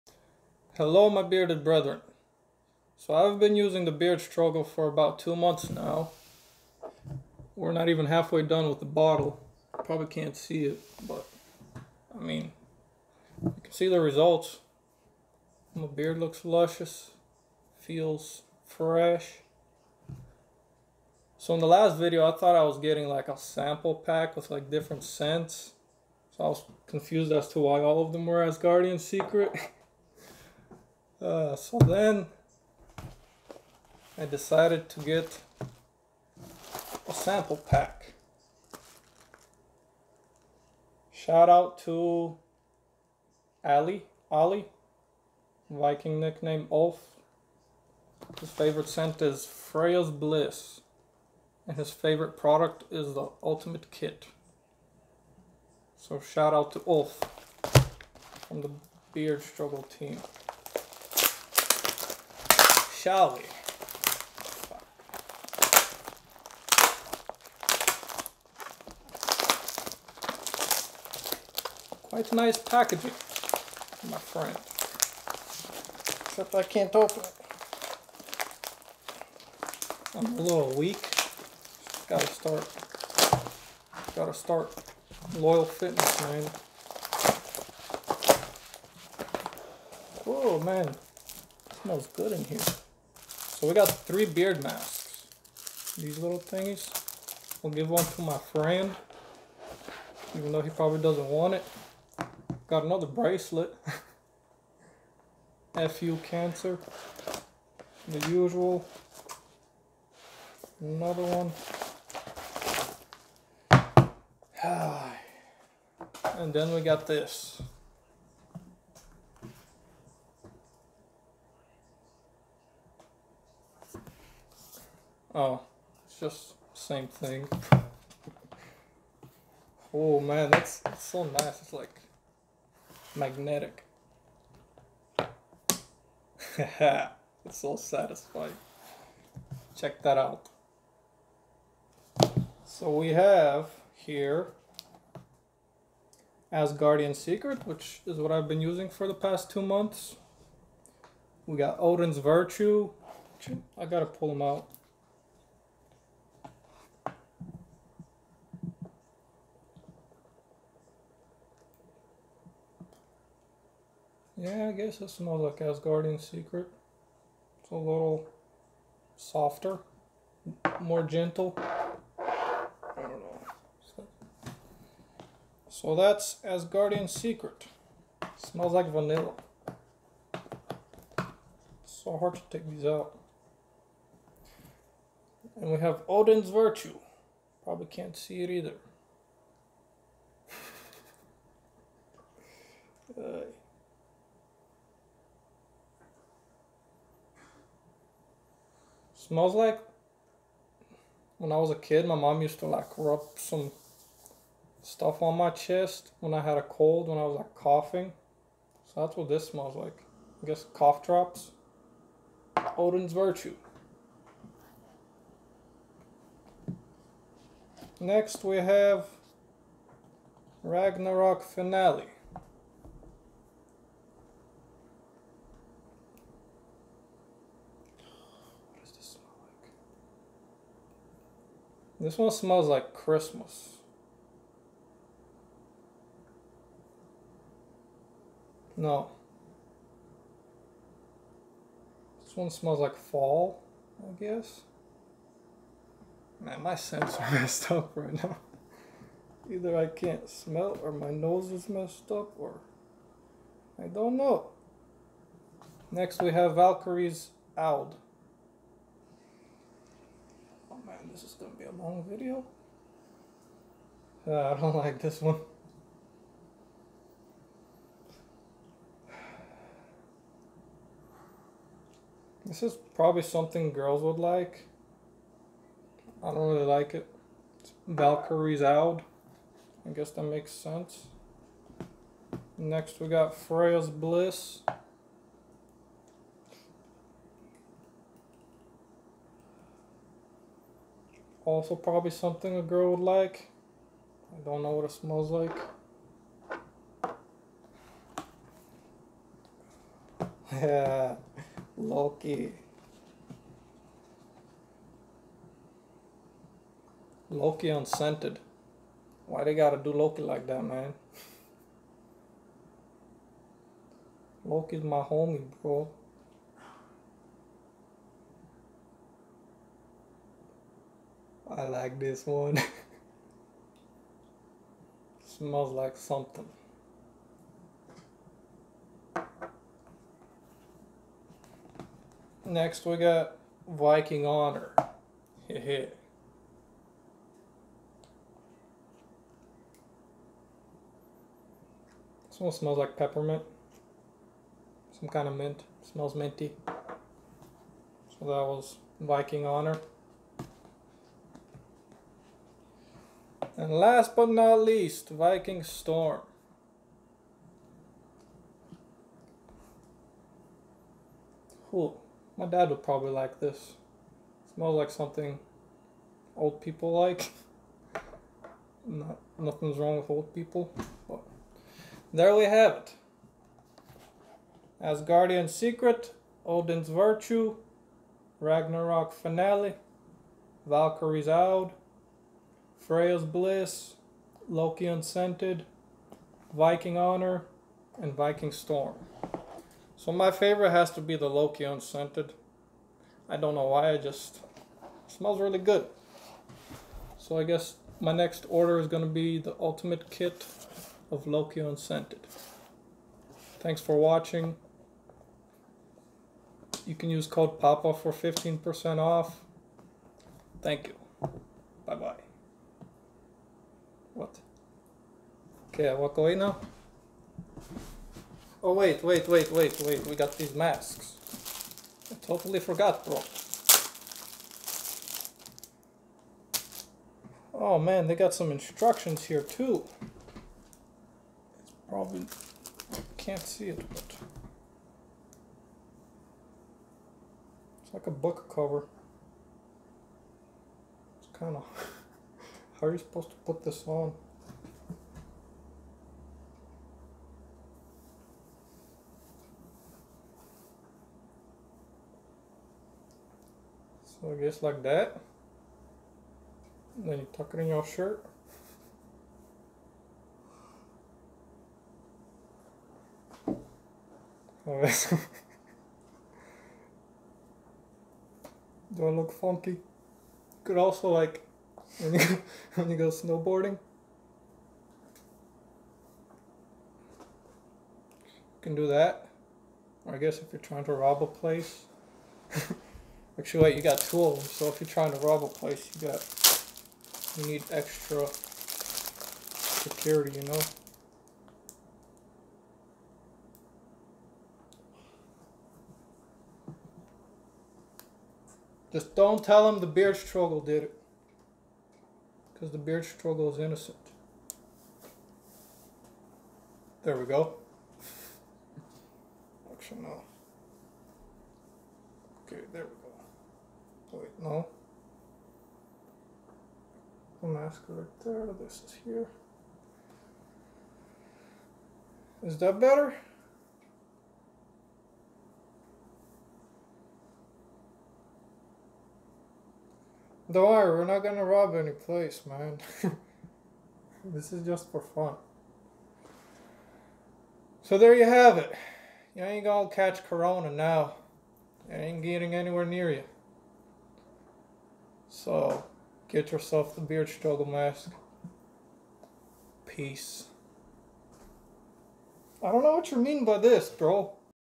The Beard Struggle Scent Sampler Review - TBS Sample Pack Unboxing ~8 Unique Scents!~
Hello fellow bearded brothers! I am Papa Ruski and today I'd like to review the product I've been using on my beard, "The Beard Struggle". It's been about 2 months now and I've been loving the results. I've been using the "Asgardian Secret" kit, which has to be one of my favorites. Now that I'm almost half done with this scent, I decided to explore the rest so I got the "Scent Sampler Pack". Watch the video and let me know which one sounds like one you'd like. Also share what the scents remind you of!

Thanks for spending some time on this channel! If you'd like to see more beard related stuff, check out my Instagram @paparuski96!

~Scents in this package~
-GOLD KIT-
1. ASGARDIAN SECRET - A woodsy intricate blend of sandalwood, vanilla, and bergamot.
2. ODIN'S VIRTUE - A sweet intricate blend of apple, cinnamon, and agarwood (oud).
3. RAGNAROK FINALE - Vanilla and patchouli base with a smoky cedarwood, and balsam earthiness.
4. VALKYRIE'S OUD - A sweet, vanylic and acrid oud scent, with sensual vanilla, and bergamot aroma.

-SILVER KIT-
1. FREYA'S BLISS - Calming ocean florally fresh essence with a hint of pine, cedarwood and lime.
2. LOKI UNSCENTED (MY FAVORITE) - Just as the name suggests, a very low key (Loki) and unscented blend.
3. VIKING HONOUR - Nuances of sweet, intoxicating bergamot and lime, with notes of peppermint.
4. VIKING STORM - A fresh, strong, masculine scent with notes of apple, patchouli, vanilla, amber, oakmoss and musk.
(8 (eight) scents total)

If you'd like to smell the scents for yourself, you could get yourself the sample pack here!